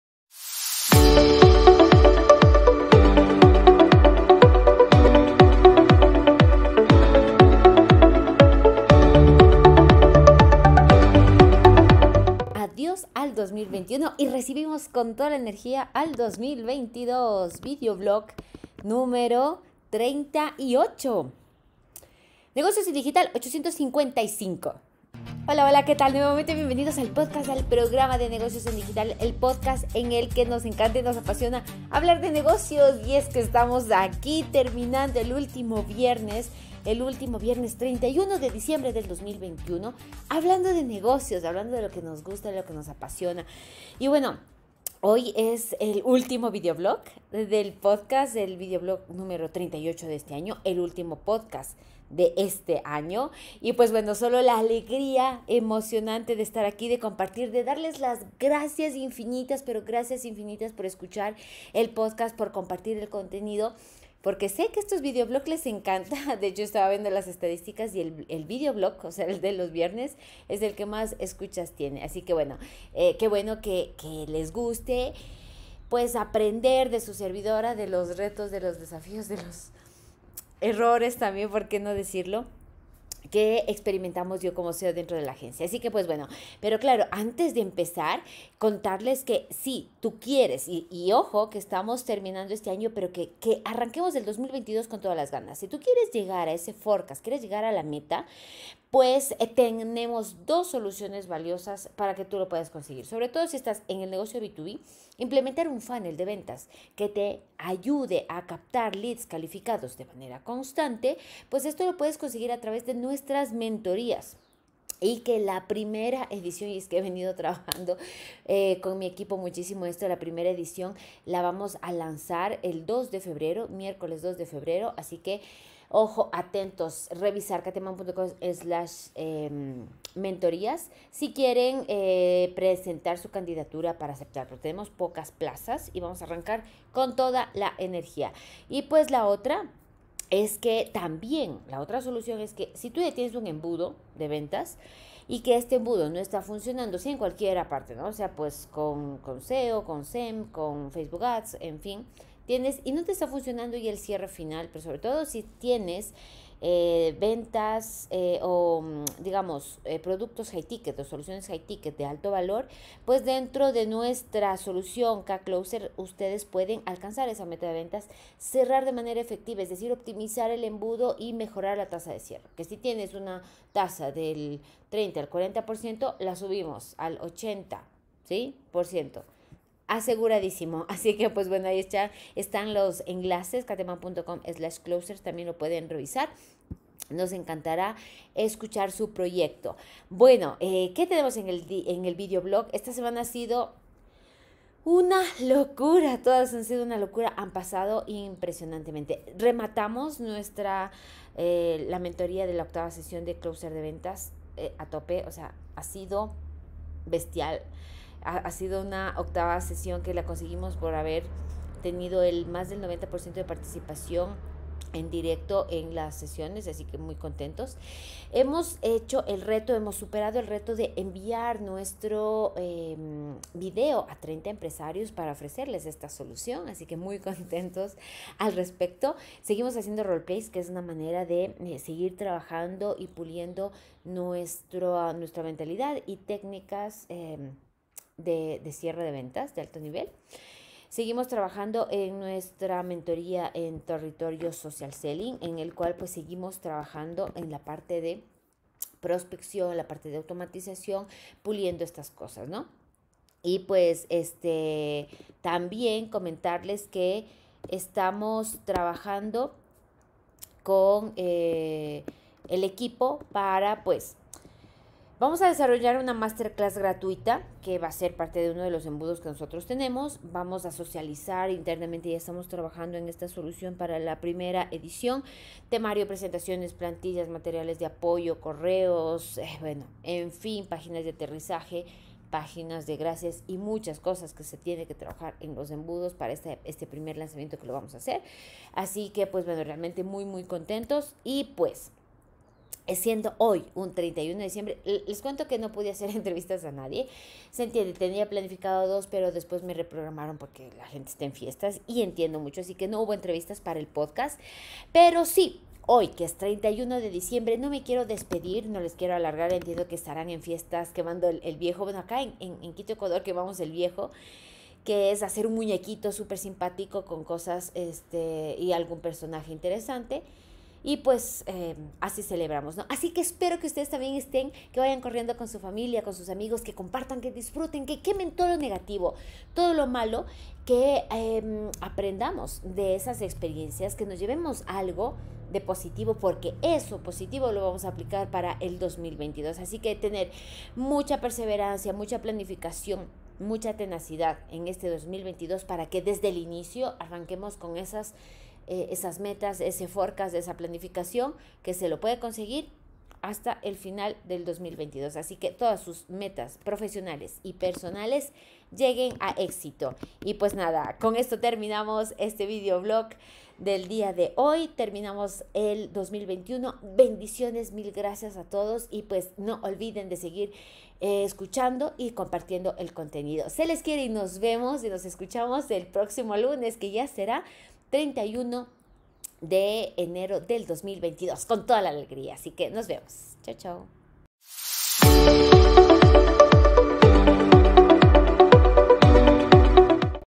855. Vlog38. Adiós al 2021 y Bendecido 2022
Último episodio del podcast del 2021 último videoblog de este mismo año. Alegres recibimos del 2022.